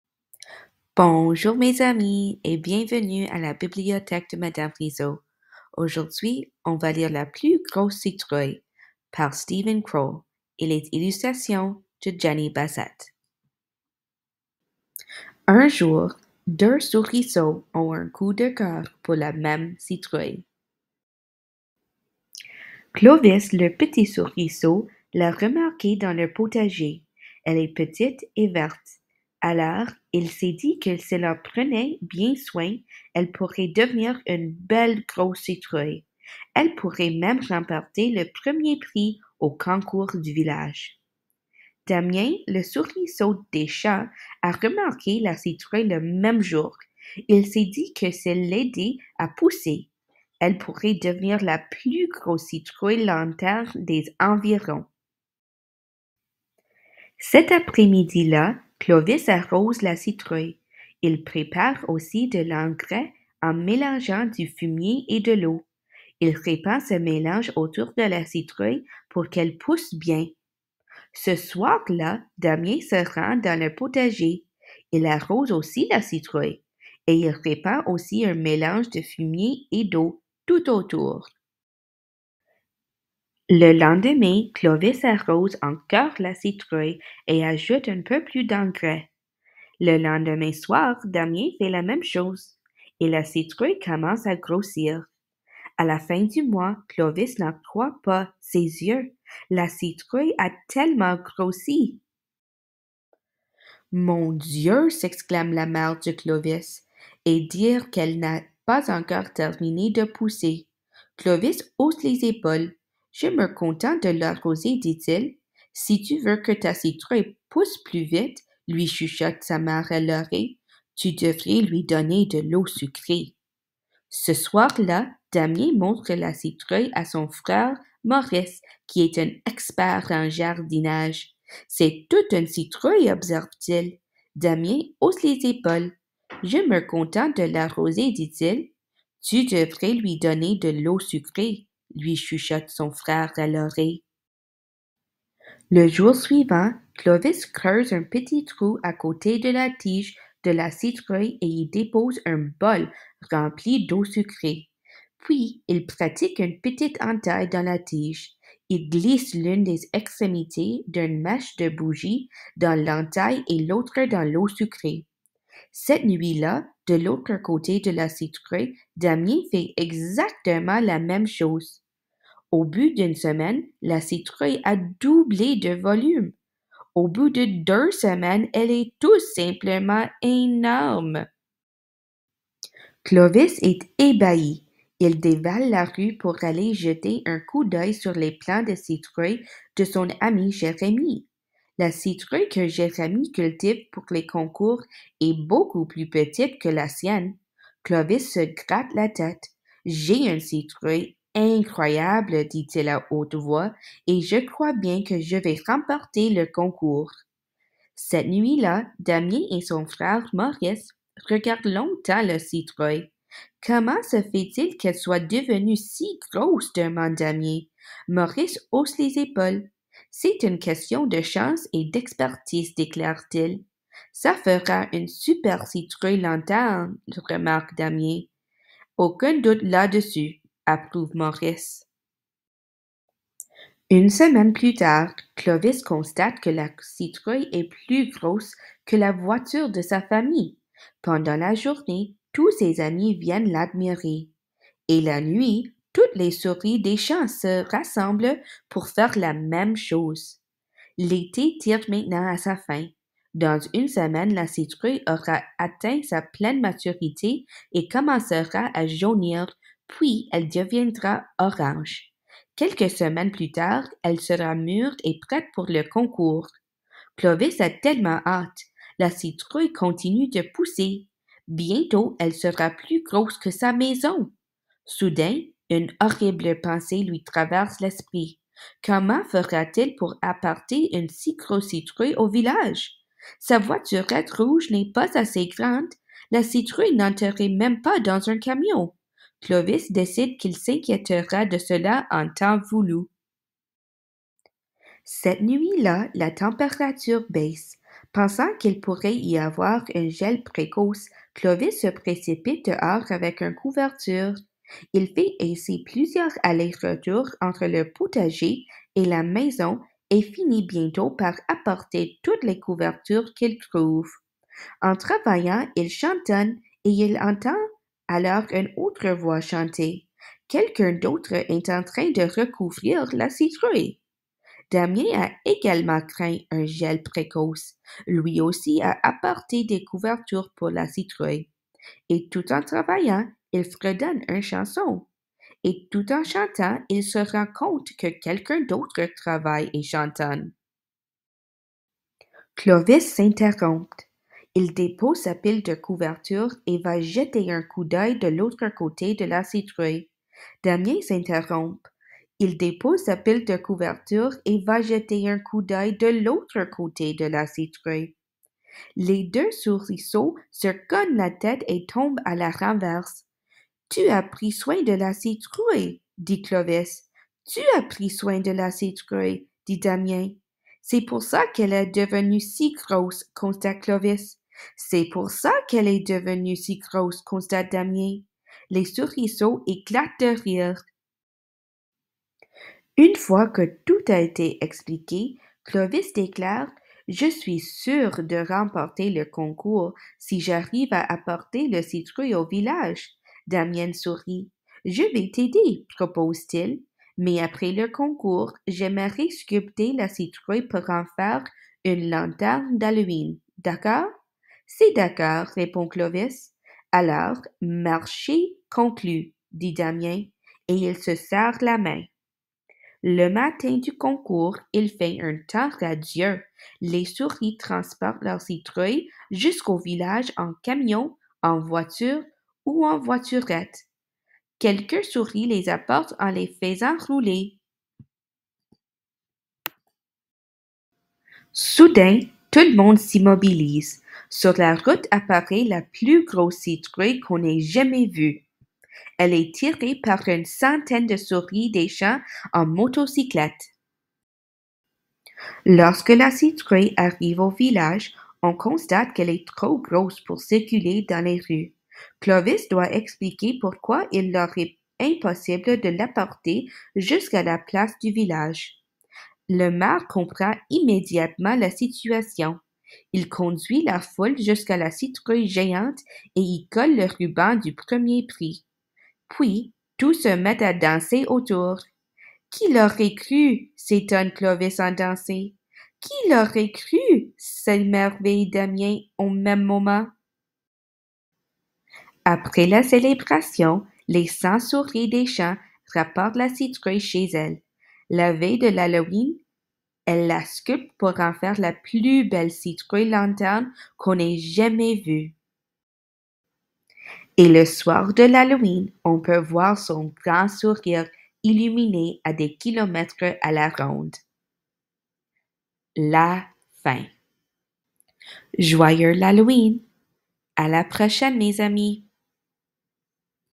📚French Read Aloud📚 La plus grosse citrouille
Bonjour mes amis!

Aujourd'hui nous lisons La plus grosse citrouille par Steven Kroll. Un jour, deux souris ont un coup de coeur pour la même citrouille. Sans le savoir, Clovis en prend soin le jour, et Damien, la nuit. La citrouille se met alors à grossir, grossir et grossir pour devenir... la plus grosse citrouille!